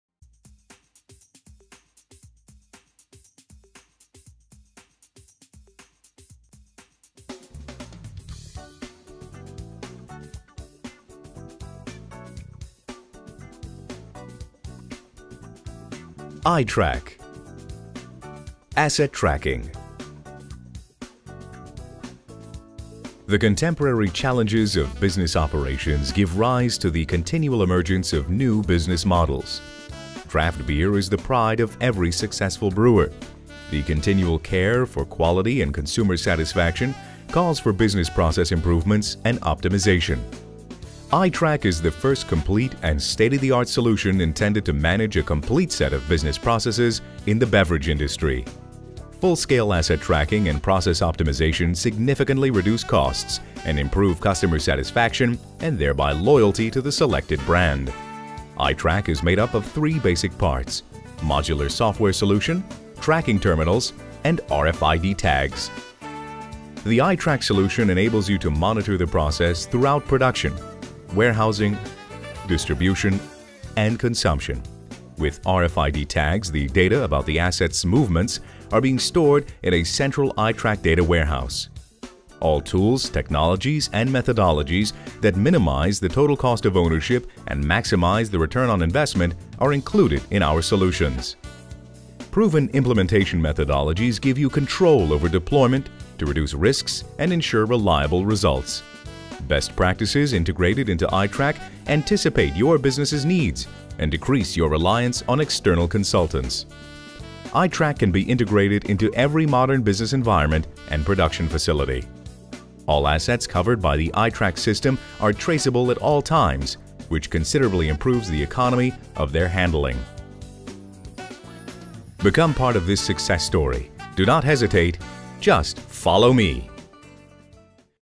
i-Track system presentation on Brau Beviale 2008 fair in Nuremberg
i-Track comprises three basic parts:

- Modular software solution
- Tracking terminals
- RFID tags

The i-Track solution enables you to monitor the entire process effectively and efficiently, from production, maintenance, and sales, to distribution and draft beer consumption. With RFID tags data about the assets' movement are stored in a central i-Track data warehouse. Their true value even improves when using i-Track application software integrated into the existing ERP system.

Proven implementation methodologies provide control over deployment in order to reduce risks and ensure reliable results. The best practices integrated into i-Track anticipate your business needs, reduce costs, and give you fast ROI (return on investment).

Tracking beverage dispensing and cooling equipment, as well as the life cycle of beer kegs and casks has always been essential. By using i-Track it becomes full scale and highly efficient.

Modern breweries have tens or hundreds of thousands of beer kegs, casks, and bottle crates in circulation; managing such large investment flows successfully keeps customers satisfied and allows you to have a cutting-edge advantage over your competition. From now on beer kegs, casks, and bottle crates can be traced more precisely and transparently than ever before.

i-Track improves draft beer quality, error frequency drops to a minimum, and stocks of beer kegs are optimized. i-Track can be integrated into every modern business environment and production facility.